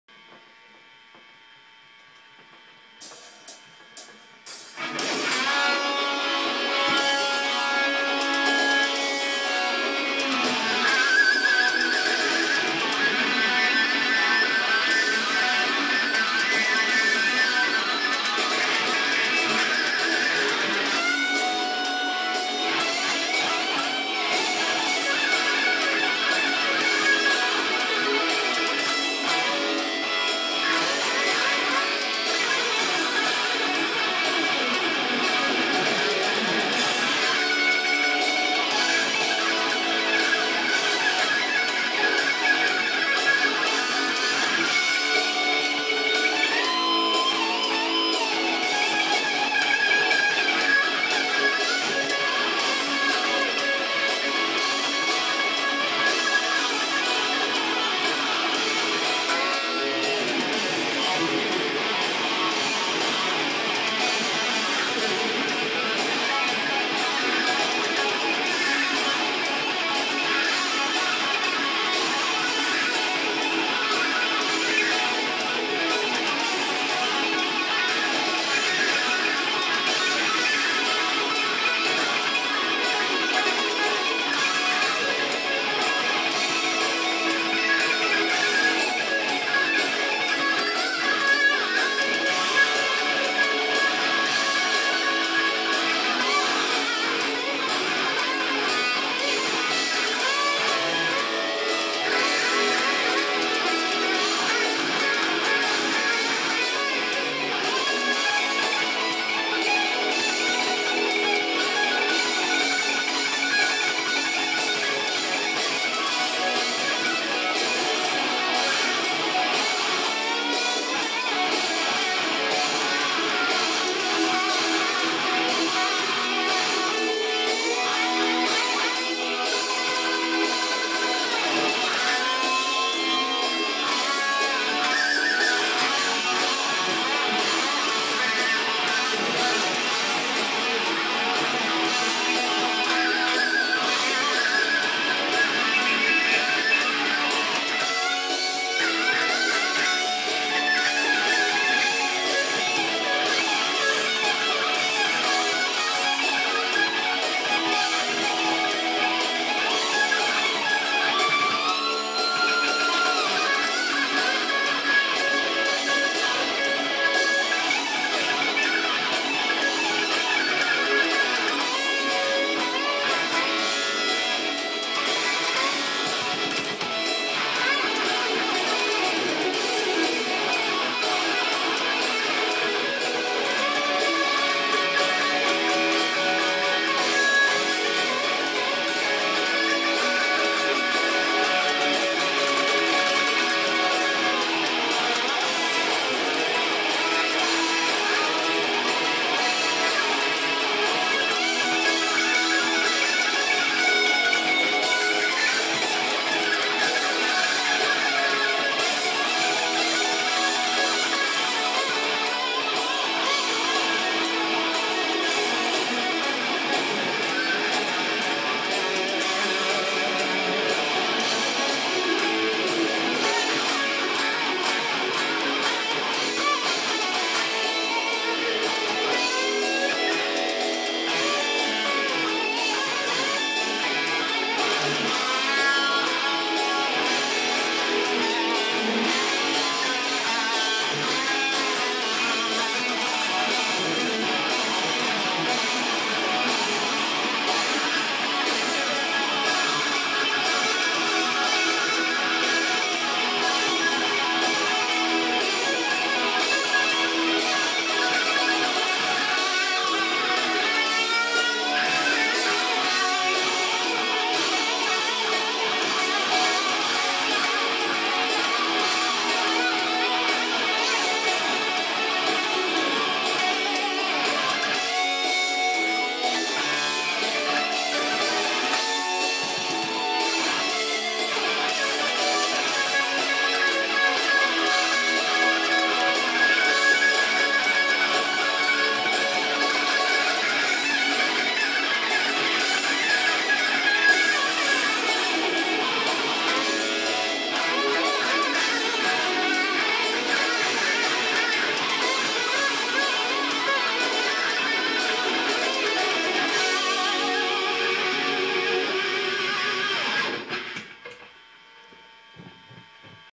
Zakk wylde style solo in f#
Jamming to a zakk wylde backing track in f# you can find it The gear used is Dean thoroughbred namm 2013 USA model Dmt nostalgia pickups in the bridge and neck Mxr berzerker overdrive pedal Cry baby wah pedal.                                             Fender mustang 3 amplifier